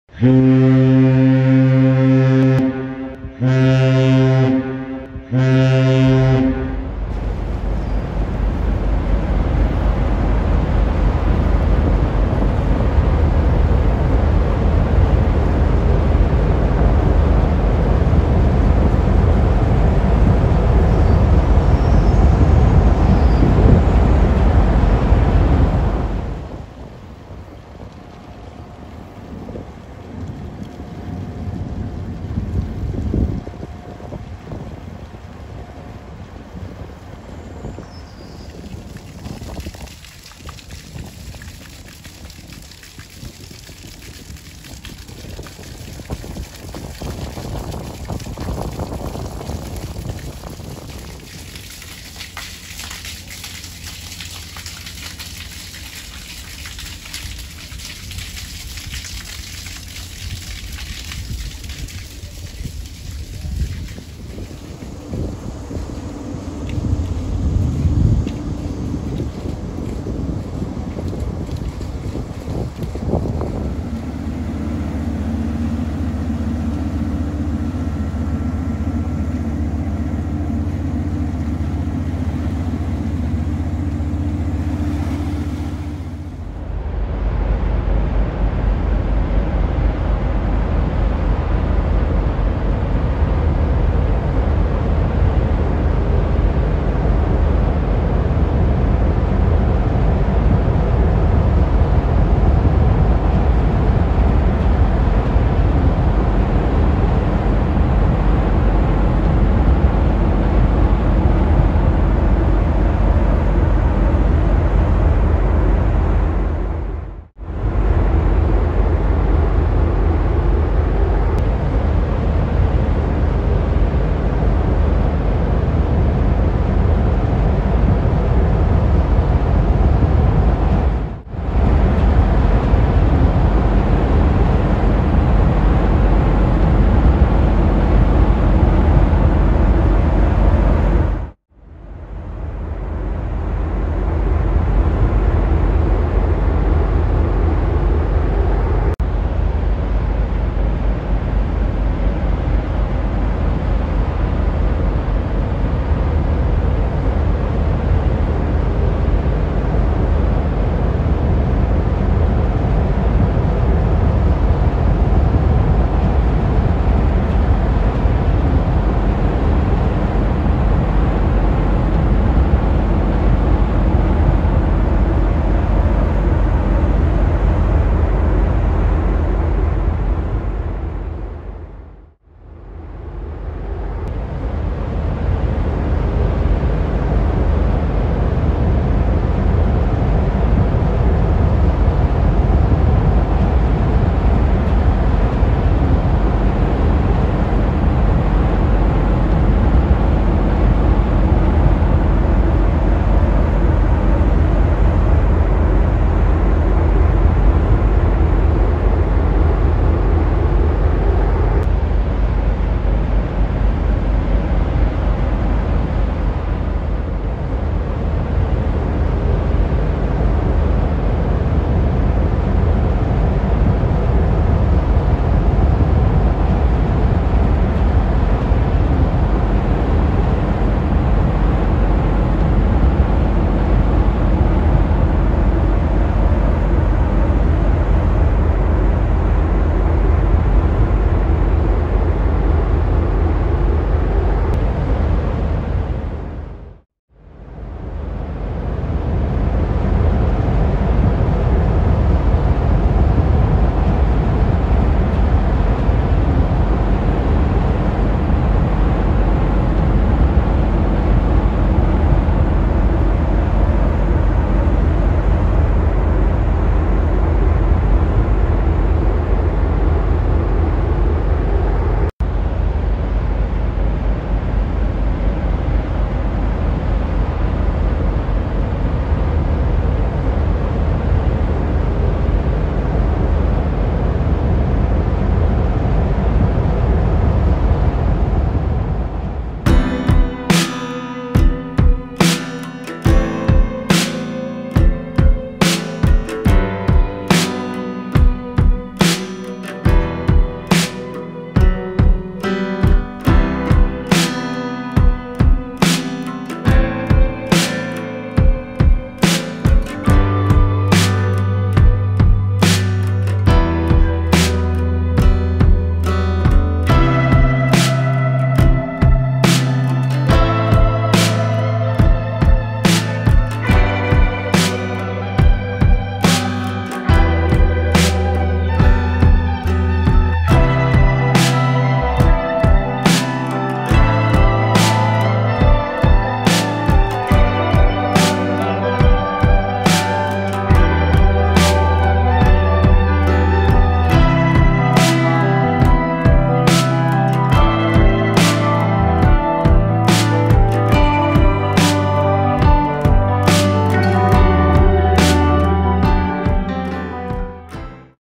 The Trillium Class Vessels of Canada Steamship Lines on the Welland Canal
The Trillium class is a series of freighters owned by Canada Steamship Lines (CSL). The class is divided into three sub-classes; the self-discharging lake freighters, the lake bulk carriers, and the Panamax self-discharging bulk carriers. Initially a nine-ship building program, six are operated by Canada Steamship Lines for use on the Great Lakes. In this video we will focus on those  six vessels:

BAIE COMMEAU
BAIE ST. PAUL
THUNDER BAY
WHITEFISH BAY

CSL ST-LUARENT
CSL WELLAND